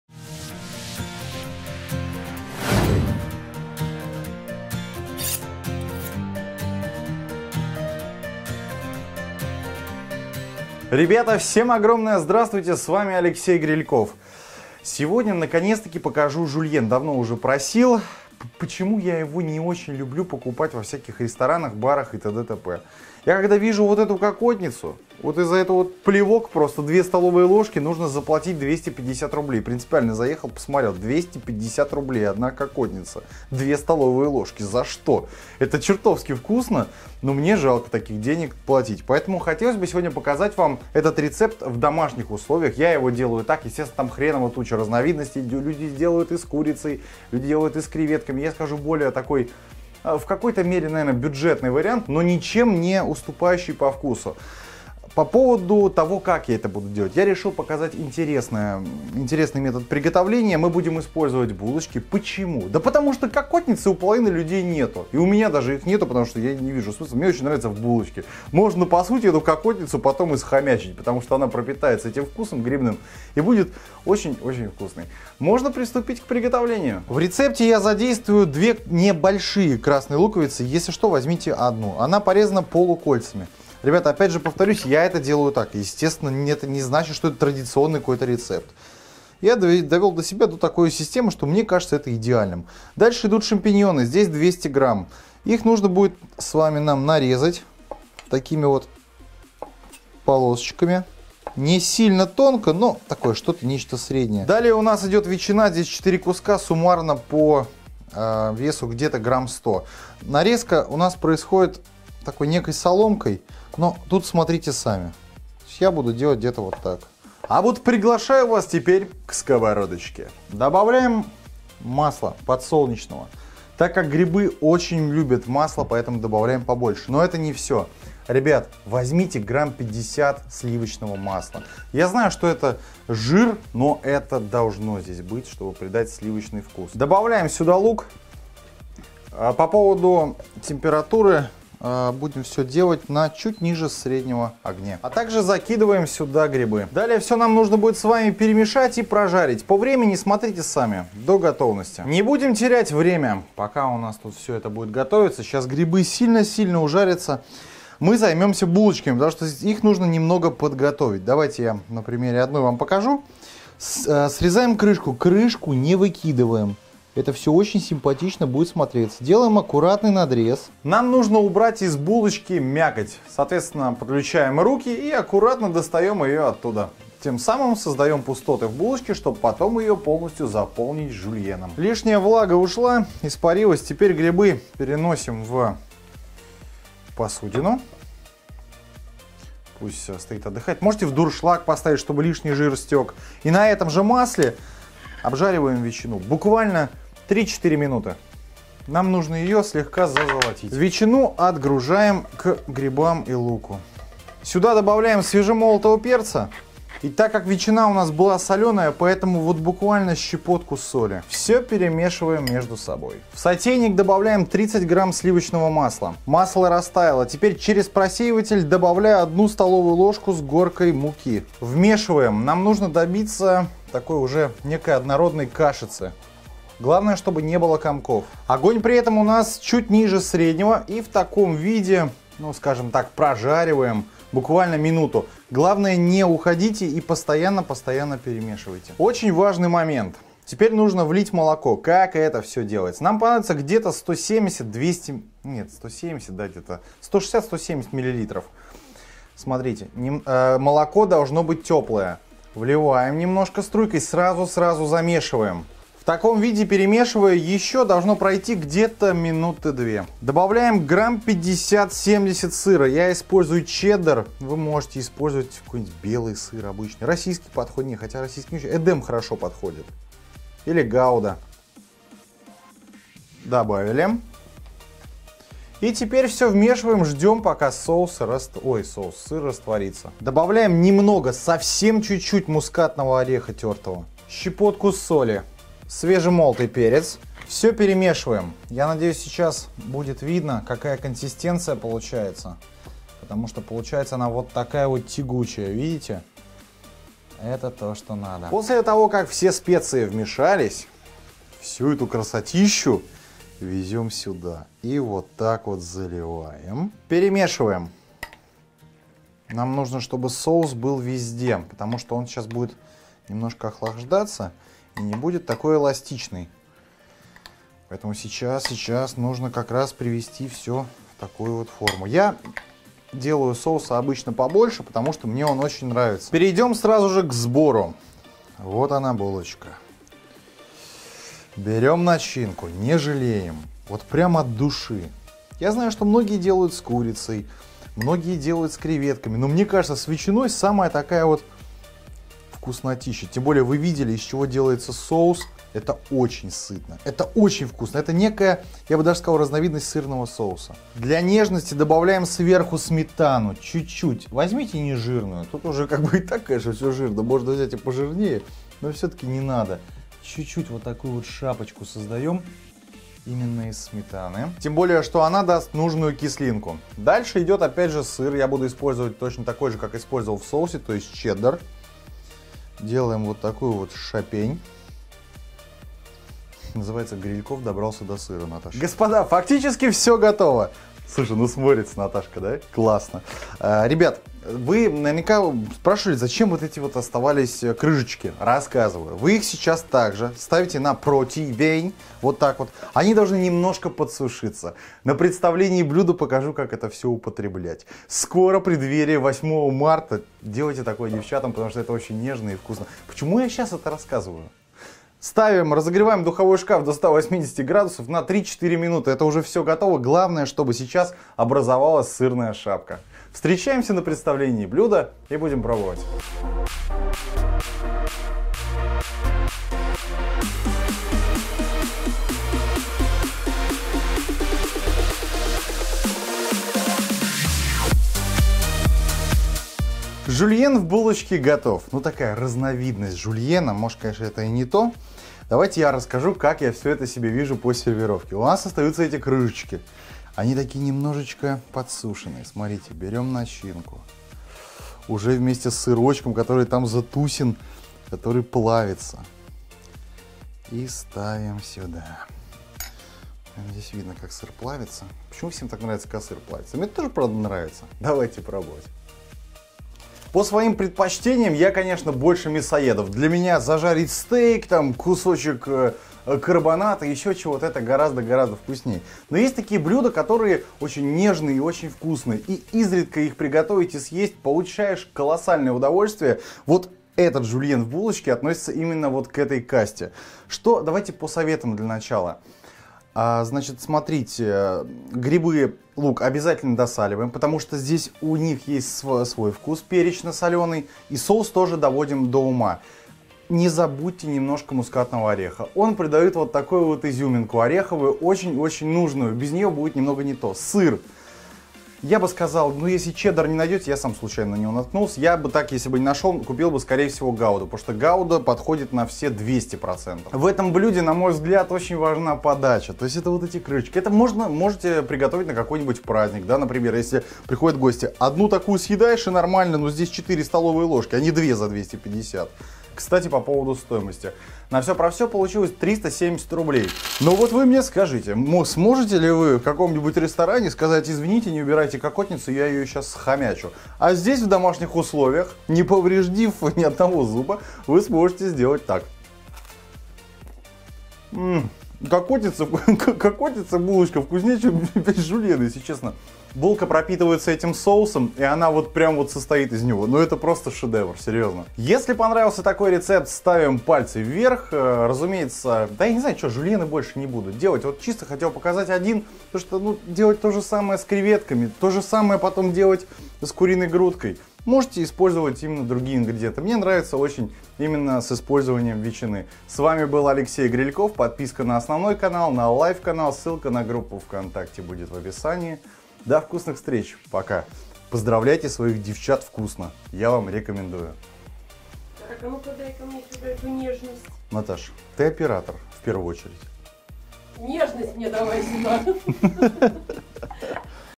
Жюльен в булочке (Не классический рецепт)
Ингредиенты:
Булочки – 4шт.
Лук -2шт.
Шампиньоны – 200гр.
Ветчина – 100гр.
Сл. Масло – 50гр.
Соль, перец – по вкусу
Соус:
Сл. Масло -30гр.
Мука – 1ст.л.
Молоко – 160мл.
Мускатный орех – 1\3ч.л.
Сыр – 70гр.
Сметана – по вкусу